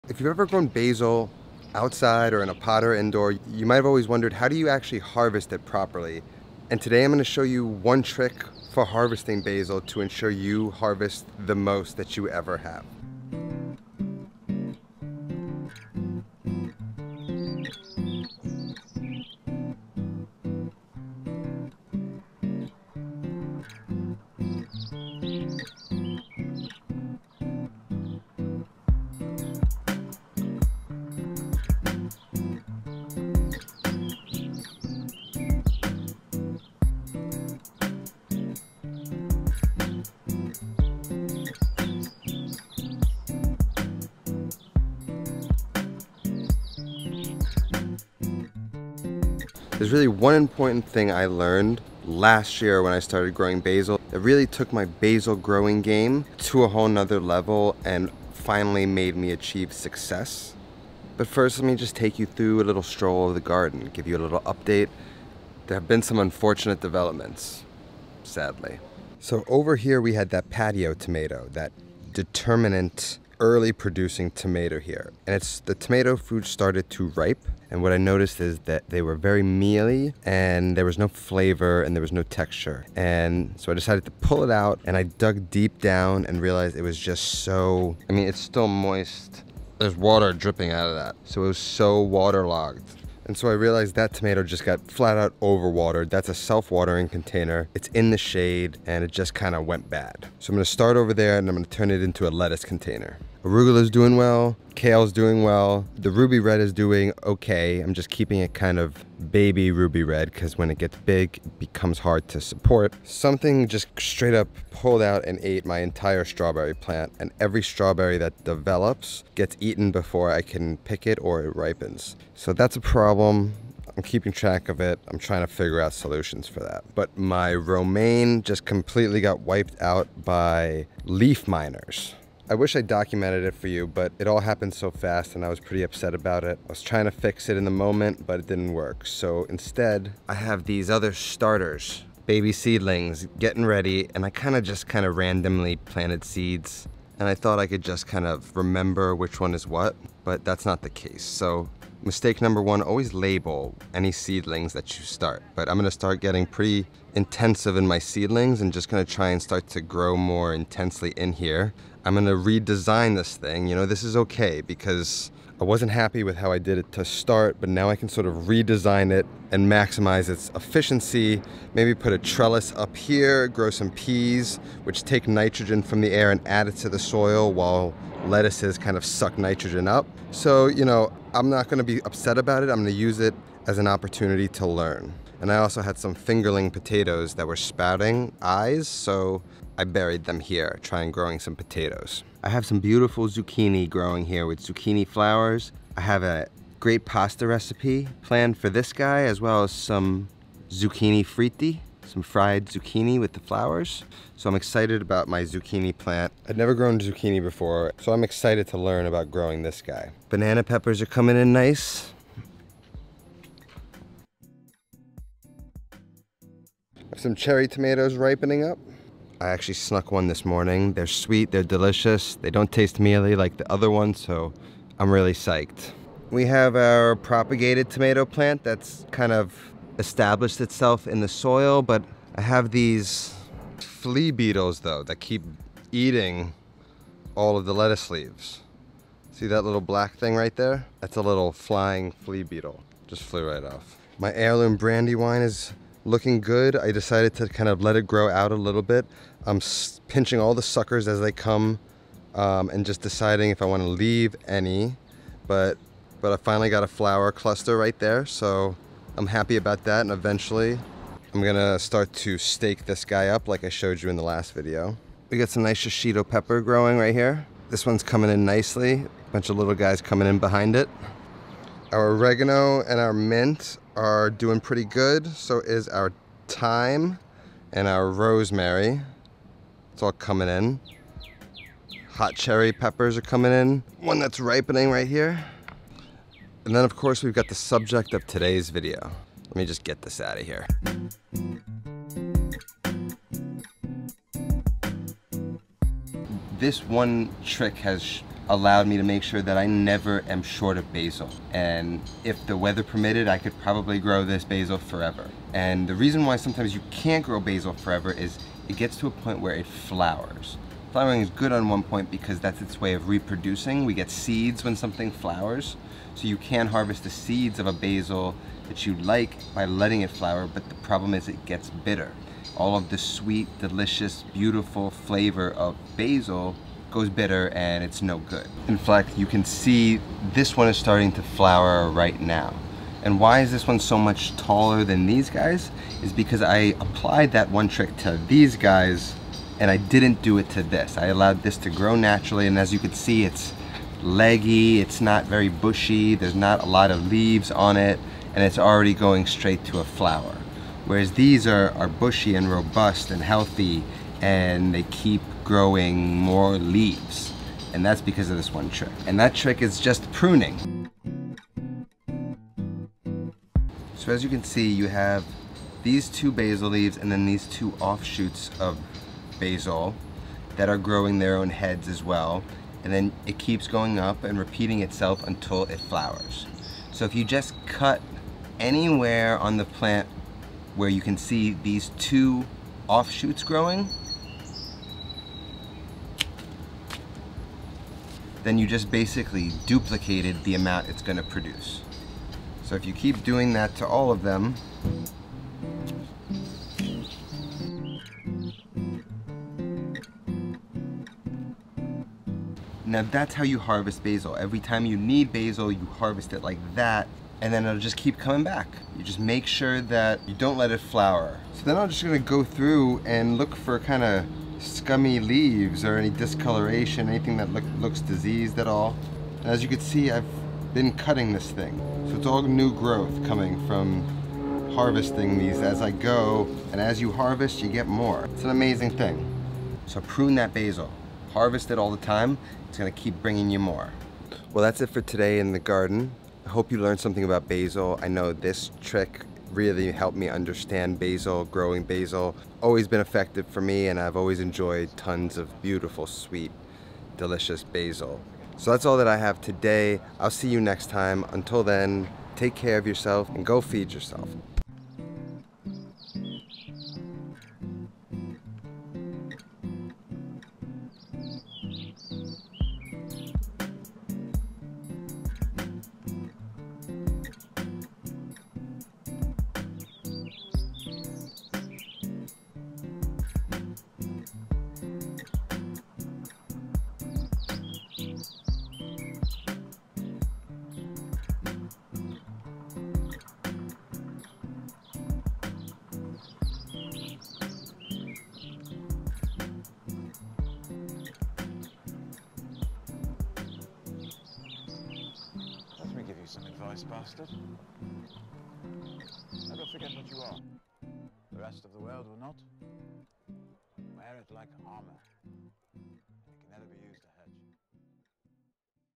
GROW THE MOST BASIL you ever have with this 1 EASY GARDEN HACK
Today I share an amazing garden hack or garden technique for growing and harvesting the most basil you ever have. Its a really easy method for pruning that will increase your basil yield substantially.